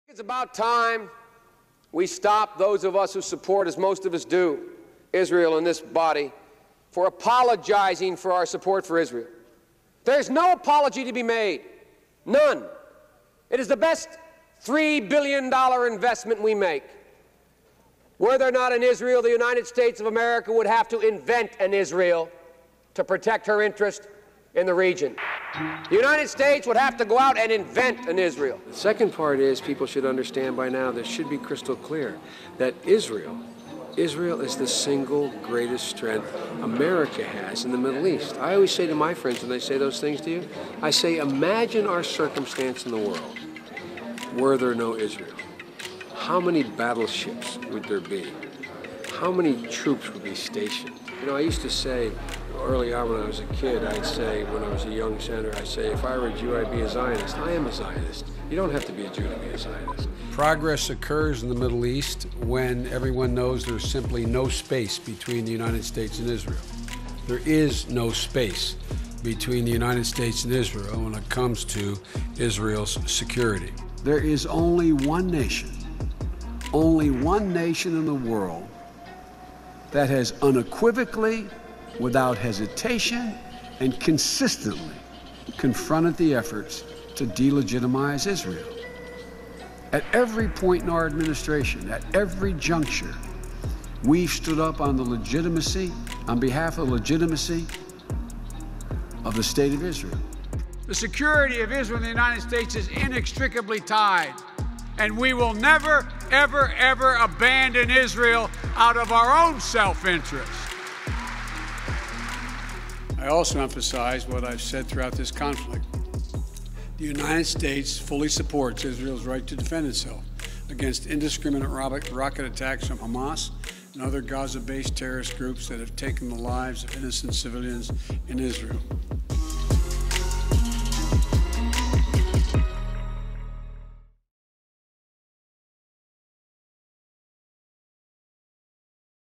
Joe Biden's long history of pro-Israel statements
"Were there not an Israel, the United States of America would have to invent an Israel."

Here's a history of surprising statements US President Joe Biden has made about Zionism and Israel.